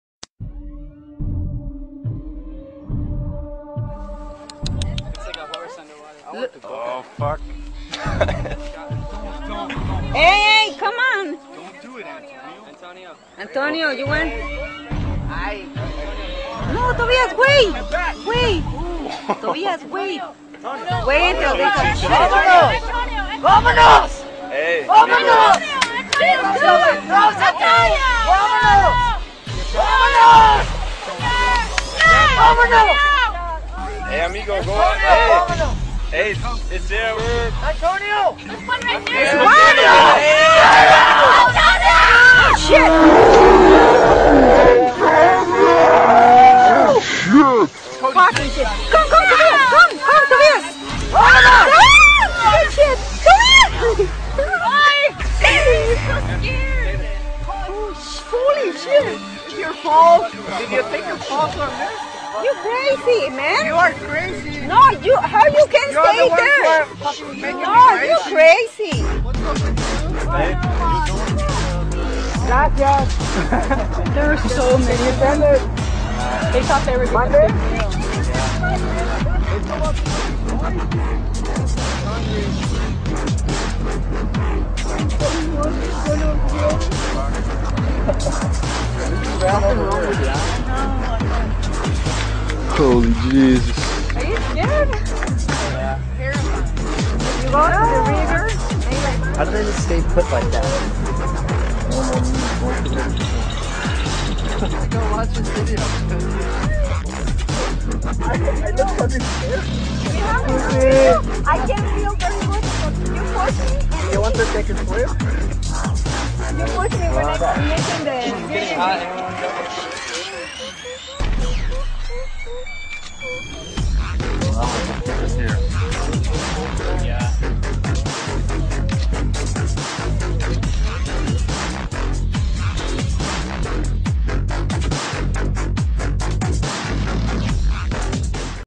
Antonio - Do not risk your life with crocodile....!
បុរស​ម្នាក់​ស្ទើរតែ​អស់​ជីវិត ខ​ណៈ​ដែល​កំពុង​ផ្តិត​យក​រូបភាព​ក្រពើ​ក្នុង​ទន្លេរ Tarcoles (វីដេអូ)